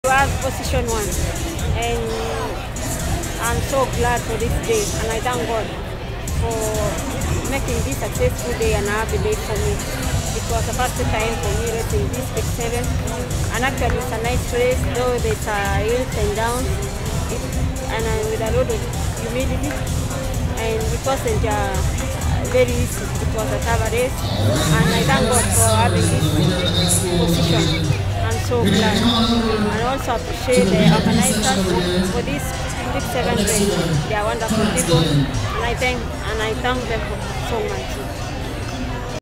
QCT Bix 7 winner Cynthia Limo
discusses her race today and the push that allowed her break out of a pack of five runners in the final mile to win